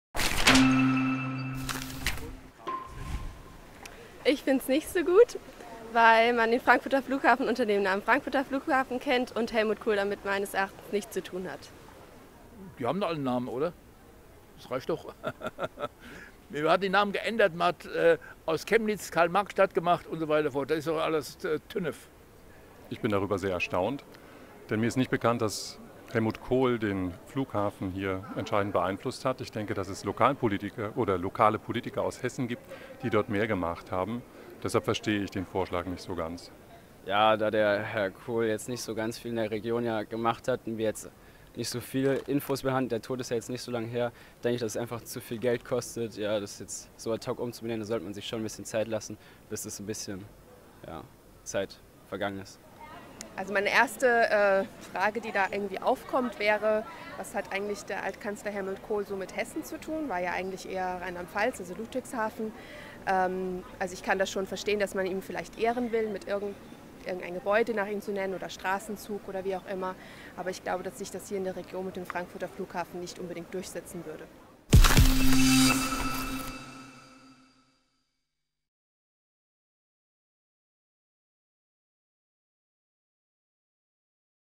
"Flughafen Helmut Kohl"?
Passantenumfrage in Darmstadt zu dem Thema: Soll der Frankfurter Airport in "Flughafen Helmut Kohl" umbenannt werden?